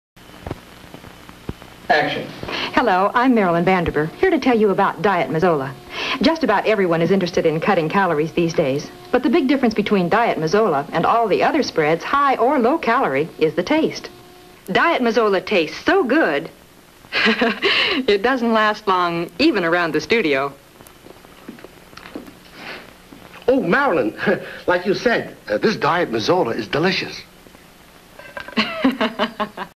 Diet Mazola margarine, television commercial (Sid Raymond’s Commercials, no. 83)
Diet Mazola margarine, no. 83 in a series of commercials starring Sid Raymond, featuring Marilyn Van Derbur.  See below for more information.